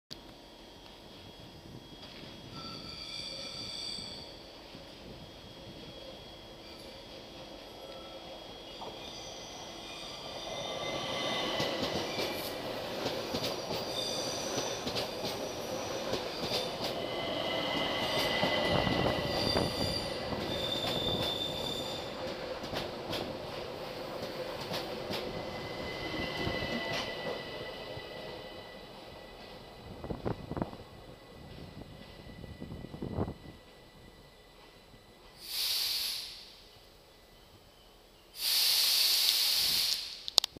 ÖBB 4124 037-5 + 4124 012-7 Cityjet arrived at Sopron (Hungary), 24. 05. 2018. [FULL HD]
ÖBB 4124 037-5 + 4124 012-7 Cityjet arrived at Sopron (Hungary), with number 7622 Bratislava-Petržalka (Slovakia) - Vienna (Wien) - Ebenfurther (Austria) - Sopron (Hungary) - Deutschkreutz (Autria) Regioexpress-train, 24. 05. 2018.
ÖBB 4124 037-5 + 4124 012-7 Cityjet érkezik Sopronba a 7622 számú Pozsonyligetfalu - Bécs - Ebenfurth - Sopron - Sopronkeresztúr Regioexpresszel, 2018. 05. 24-én.
By: Bence Petrás